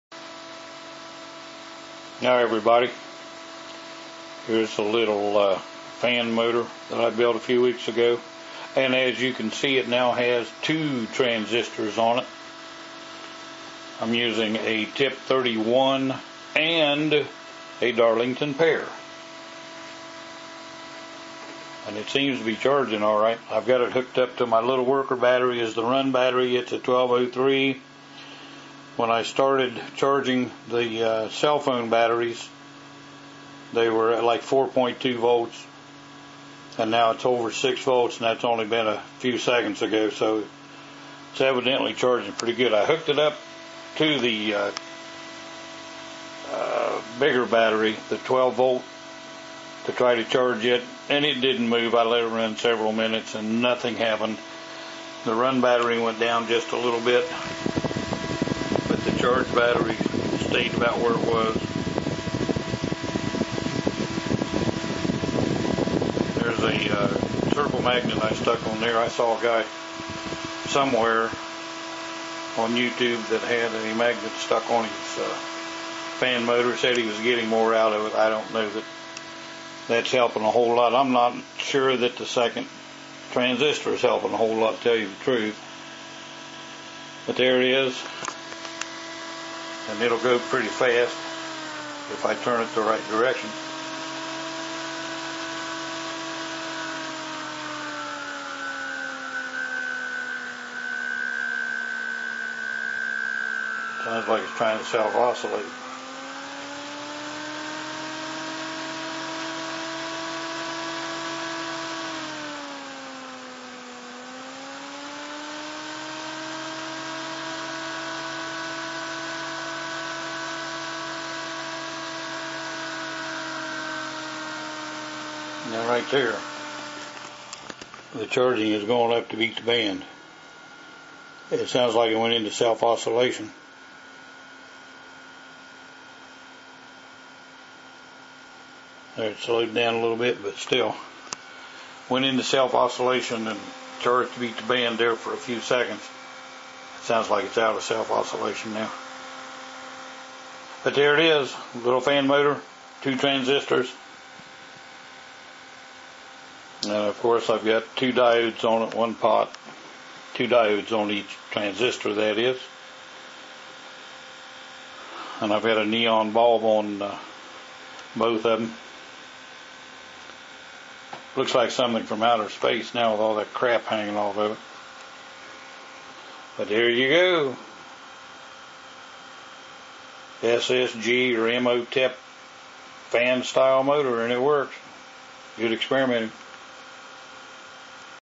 SSG Fan With Darlington Pair.avi
Imhotep fan motor using Darlington Pair (TIP142) and TIP31 Transistors.